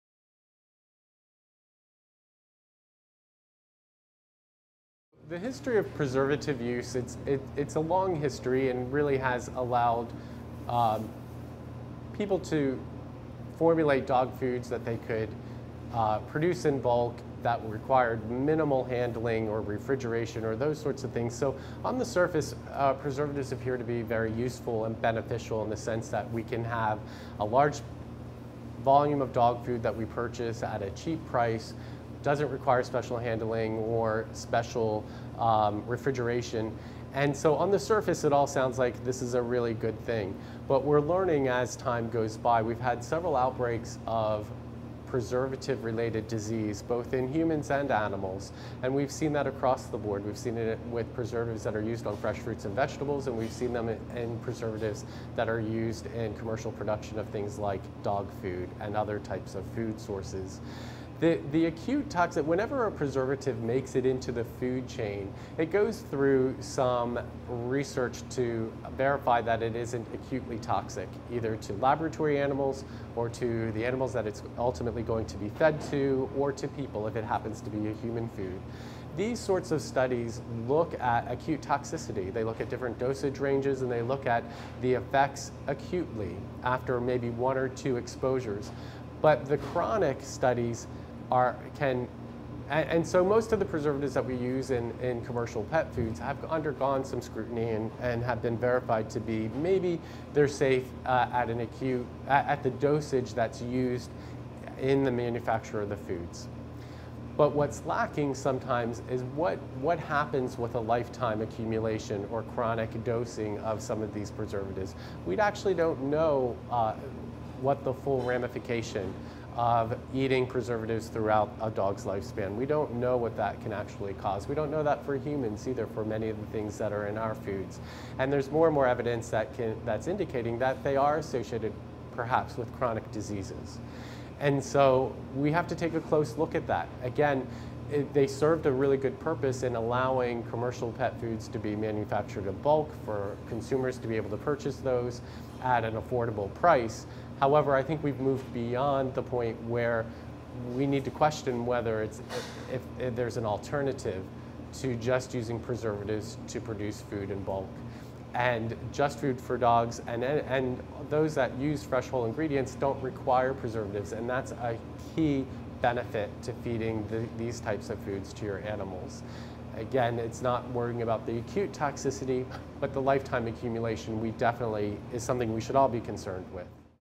The Specialists: Preservatives in Dog Food
Dr. John Tegzes,VMD, Diplomat American Board of Veterinary Toxicology from Western University of Health Sciences discusses why preservatives are used and what we know and don't know about their effects over time in both dogs and humans.